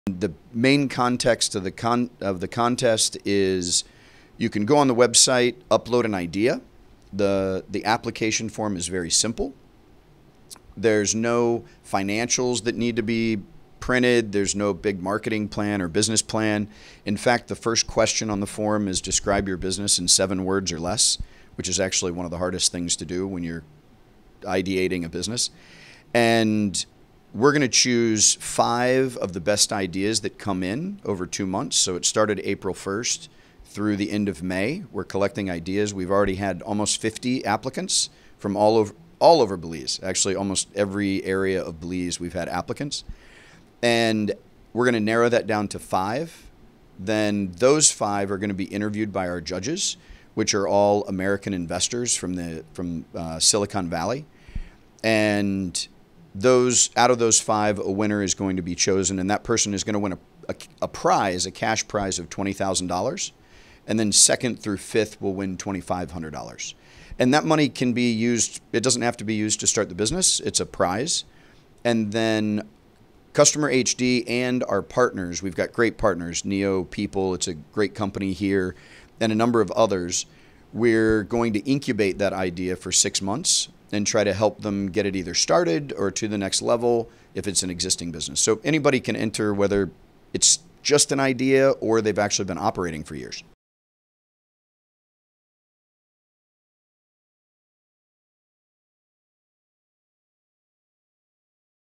Customer-HD BPO Launches "Dream Big" Contest for Belizean Entrepreneurs | PT 1
The owners of Customer-HD BPO are encouraging Belizeans to dream big for a chance to win twenty thousand dollars. The outsourcing company has launched a competition to inspire creativity among Belizean entrepreneurs. Through a website, people can submit applications and business ideas that will be judged by a panel of US investors. The person with the best idea will then be chosen and provided with an opportunity to make their dream a reality.  Co-owner of Customer-HD BPO, Paul Schmidt, explained how you can enter the “Dream Big” contest and compete for a shot at being your own boss.